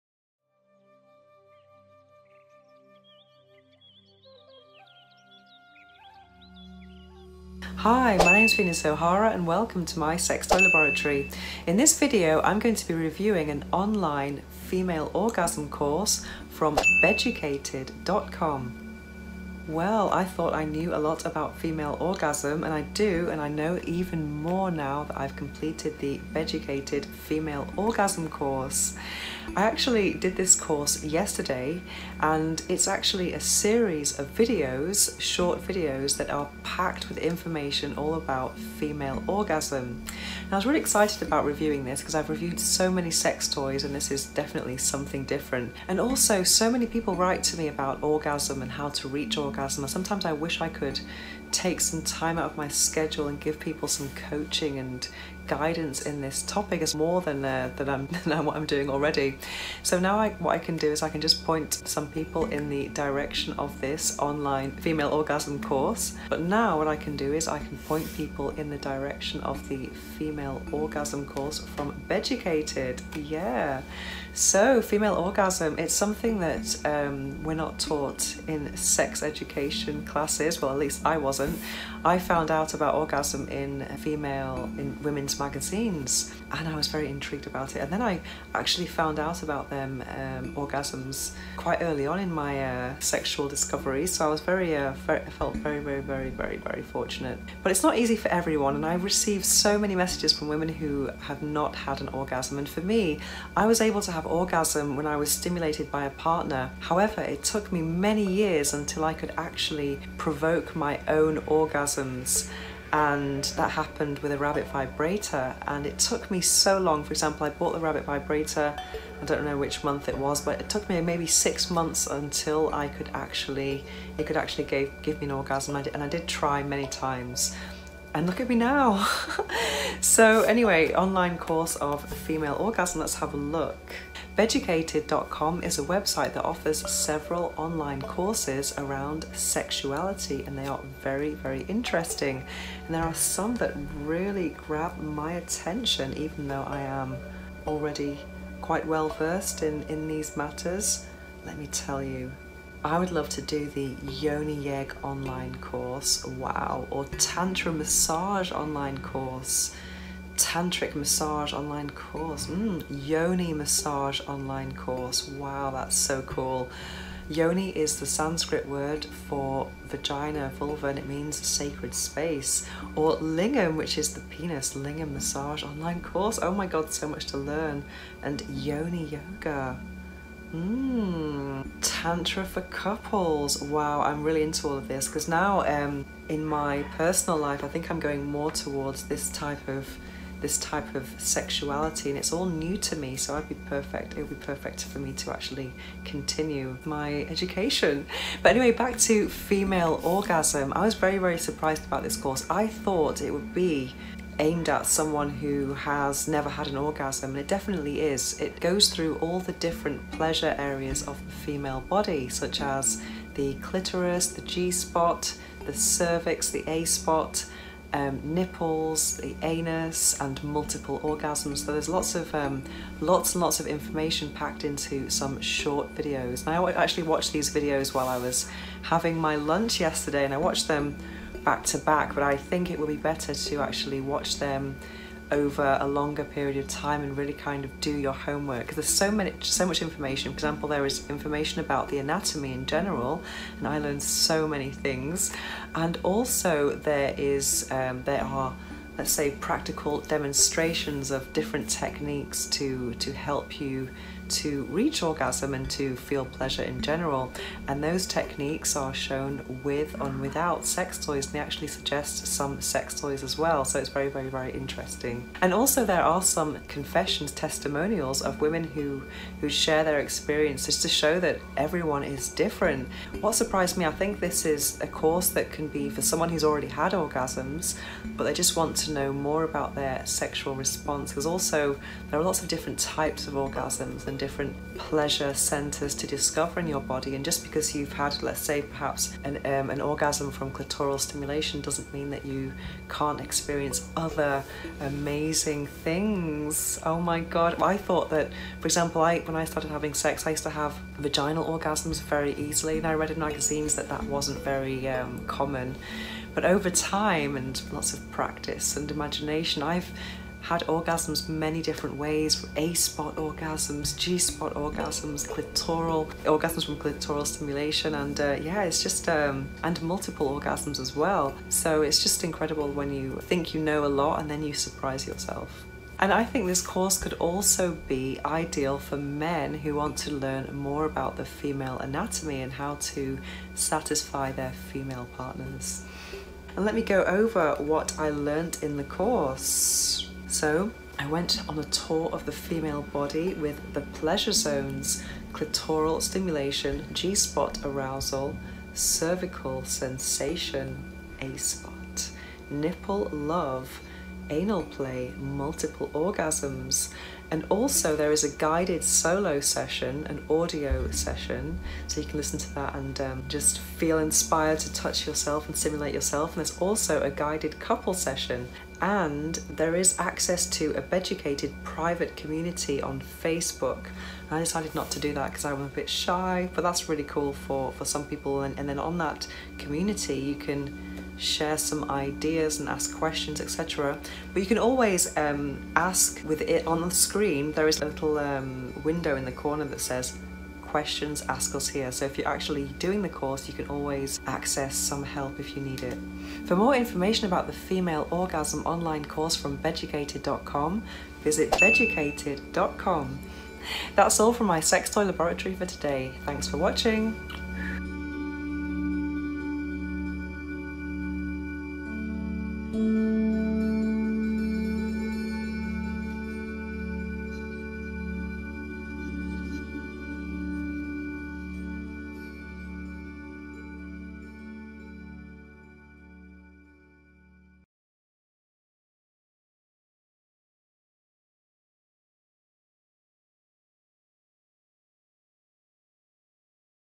Female Orgasm Online Course from Beducated.com
Think you know a lot about female orgasm? In this video, I review an online course about female pleasure on Beducated